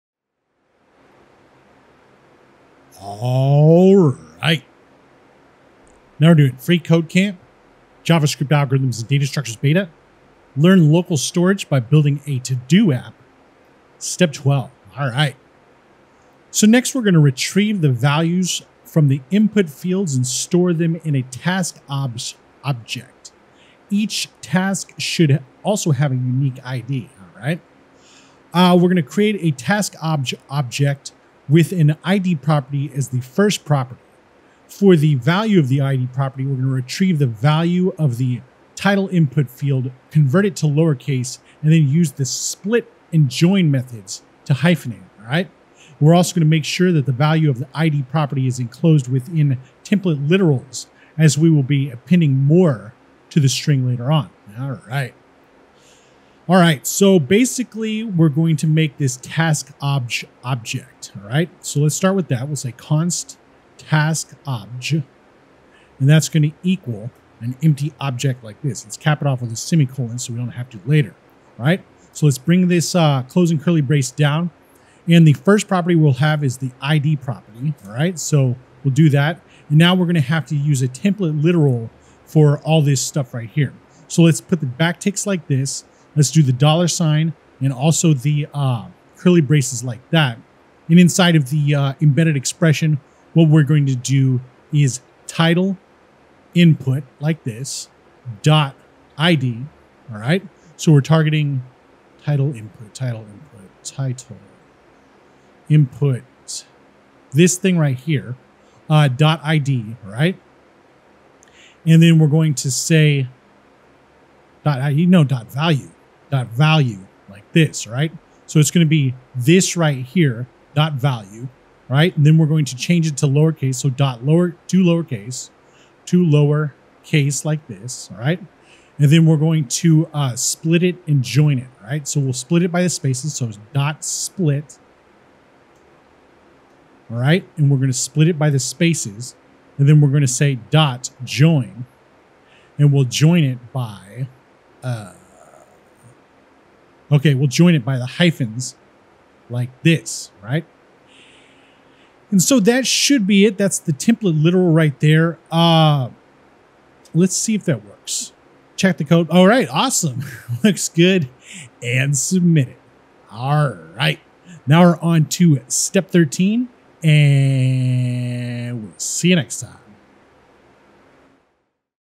Learn localStorage by Building a Todo App: Step 12 | freeCodeCamp | JavaScript | (Beta)
🌟 "Crafting Unique Tasks: Generating IDs and Retrieving Inputs in Your Todo App" 🌟

🔍 Advancing to Step 12 of our Todo app development journey, we delve into the crucial process of crafting unique identifiers for tasks and efficiently retrieving user inputs. This step is fundamental to ensuring each task within your application is distinctly recognized and managed. By creating a `taskObj` object and meticulously extracting values from the input fields, you lay the groundwork for a robust task management system. The creation of a unique `id` for each task is pivotal, serving as the cornerstone for task identification, manipulation, and storage.

The `taskObj` object is initiated with an `id` property, which is ingeniously generated from the value of the `titleInput` field. By converting this value to lowercase and employing the `split()` and `join()` methods, you transform the title into a hyphenated string, crafting a unique identifier that is both descriptive and standardized. This `id` is encased in template literals to accommodate further string manipulation or appending as needed, demonstrating foresight in data handling and object composition.

📌 Enhancing Data Organization and Accessibility:

The introduction of unique `id` properties within `taskObj` objects significantly enhances the organization and accessibility of tasks within the app. This method ensures that each task can be precisely identified, queried, and manipulated, paving the way for efficient data management and user interaction.

📊 Streamlining Task Identification:

This tutorial underscores the importance of streamlining task identification through unique `id` generation. By leveraging the content of the `titleInput` field to create a descriptive yet standardized identifier, you ensure that tasks are easily recognizable and manageable, enhancing the overall user experience.

✏️ JavaScript: Empowering Advanced Data Handling:

The process of generating unique identifiers and retrieving input values exemplifies JavaScript's capabilities in empowering advanced data handling and manipulation. This step showcases how JavaScript can be utilized to create dynamic, interactive web applications that are both user-friendly and data-centric.

📘 Advancing Your Web Development Proficiency:

As you implement this functionality, you're not just progressing through the development of your Todo app; you're also advancing your proficiency in web development. This tutorial provides a practical context to apply and deepen your understanding of JavaScript object manipulation and string handling techniques, further enriching your development skill set.

🌍 Sharing Your Progress and Discoveries:

As you navigate through the intricacies of generating unique task identifiers and handling user inputs, share your progress and any discoveries you make with the developer community. Engaging with peers on platforms like FreeCodeCamp can offer additional insights, encourage innovation, and foster a sense of camaraderie and support.

📈 Showcasing Your Development Achievements:

Successfully integrating unique `id` generation and input retrieval into your Todo app marks a significant achievement in your web development journey. This accomplishment not only enhances the functionality of your app but also demonstrates your growing expertise in creating sophisticated web applications that cater to specific user needs and preferences.

Celebrate this milestone in your project, looking forward to further challenges and achievements as you continue to enrich your Todo app with dynamic features and functionalities.